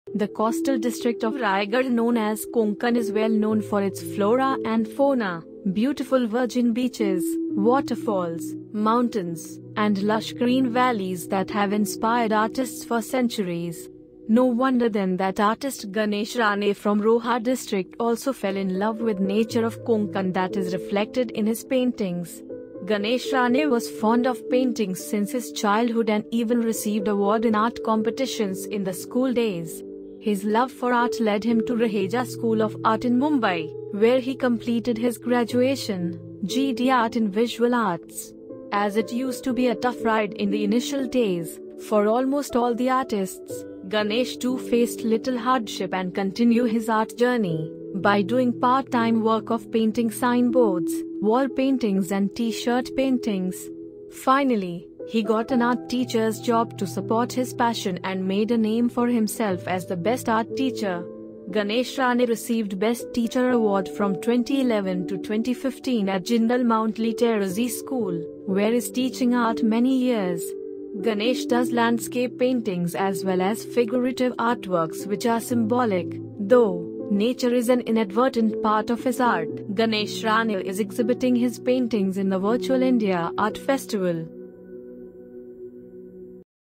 Ganesh Rane Exhibiting at Virtual India Art Fest
India Art Festival, the only modern and contemporary art fair in India that hosts annual editions Mumbai, New Delhi and Bangalore is presenting virtual edition from 01 April 2021 to 30 June 2021